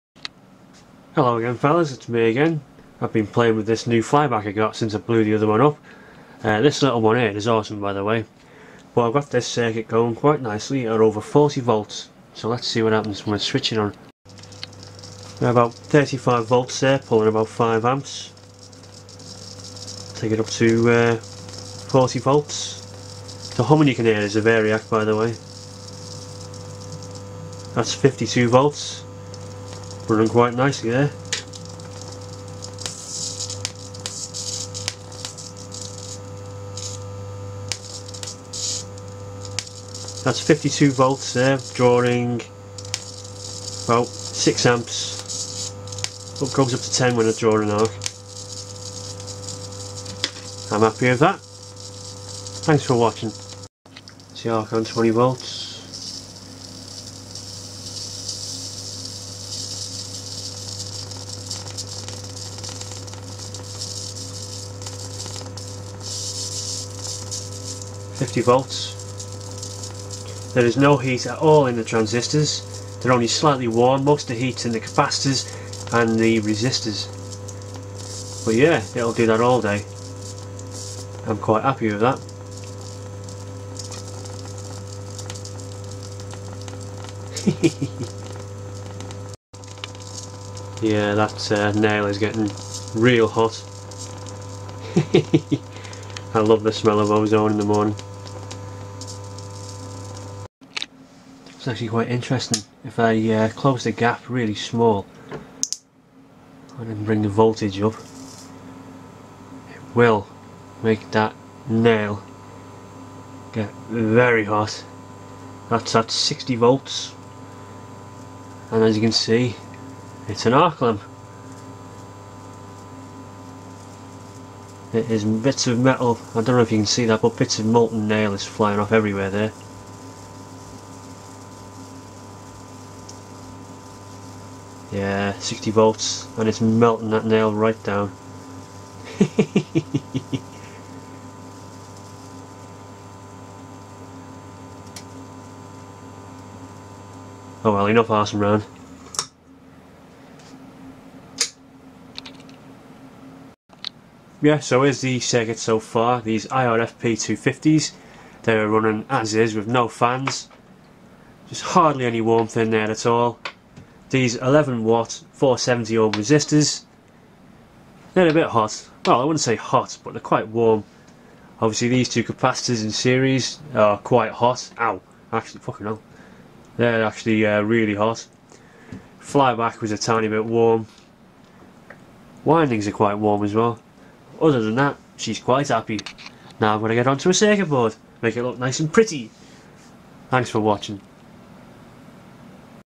Flyback Transformer: Quick update.
Got a new transformer to play with since the other one went dead short between almost all of the pins lol.
She runs quite happy without blowing up when I crank it up on the variac now.
Just need to get some mineral oil to stick this under as if I try and widen the arc gap I get a nice big spark between a couple of pins when the arc strikes even with all the hot glue.